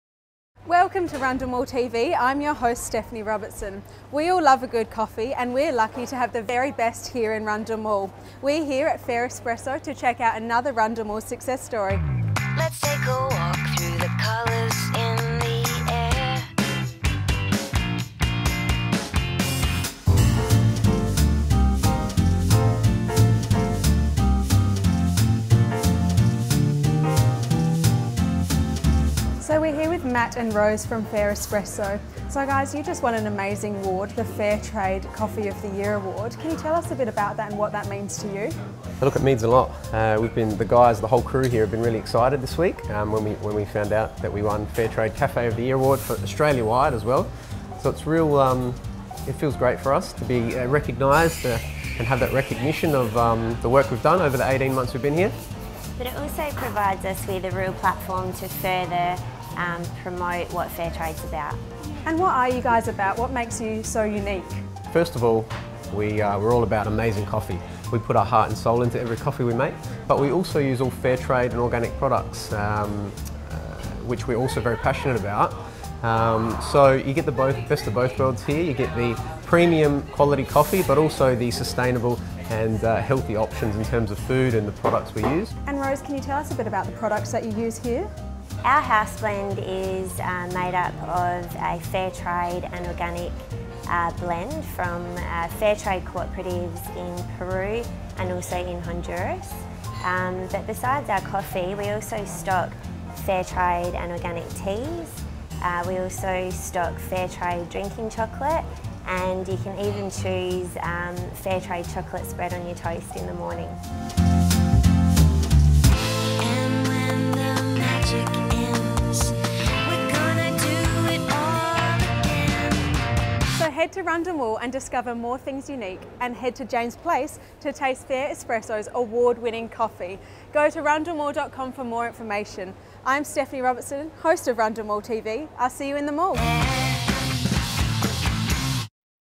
Rundle Mall TV Episode 122: Congratulations Fair Espresso!
Have you ever tried a freshly ground 100% fair trade organic coffee or hot chocolate?

Located on James Place (just off Rundle Mall), Fair Espresso has fast become a popular meeting place for city workers and shoppers to enjoy a range of fair trade products!
Follow Rundle Mall TV Host Stephanie Robertson as she visits Fair Espresso to try their delicious coffee and congratulate owners Matt and Rose on winning the National 'Fair Trade Cafe of the Year' Award!

Congratulations to the team at Fair Espresso and see you in the mall!